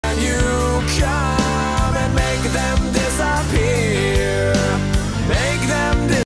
Where’d My Ball Go?
I was mowing today and just before I mowed over the basketball, this happened! Where did it go?